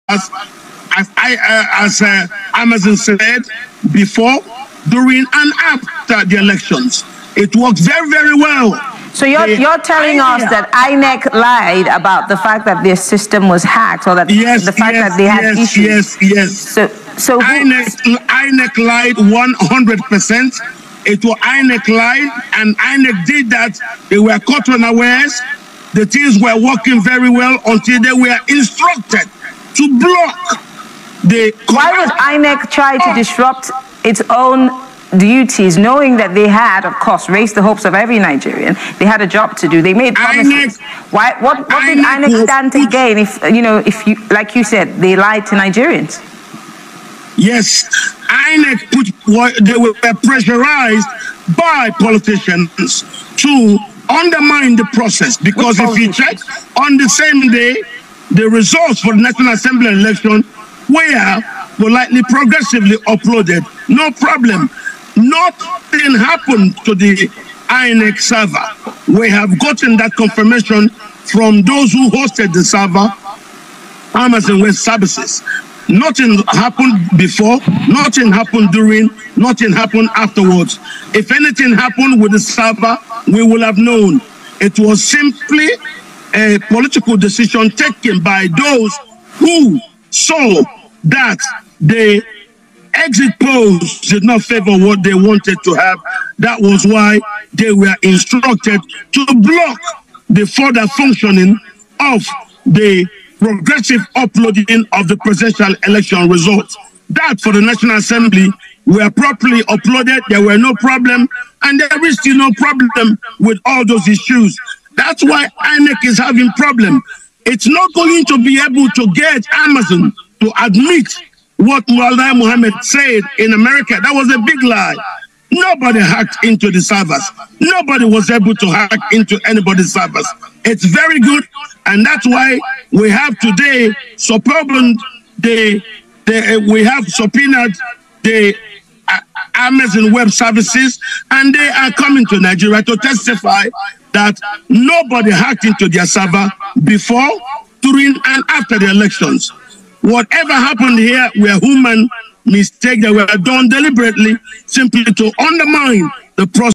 Peter Obi/LP Legal presented staff of Amazon Web Services, company INEC hired to host their servers.
The Presidential election petition tribunal today took a different turn as the Peter Obi/Labour Party Legal team presented a staff of Amazon Web Services, the company INEC hired to host their servers.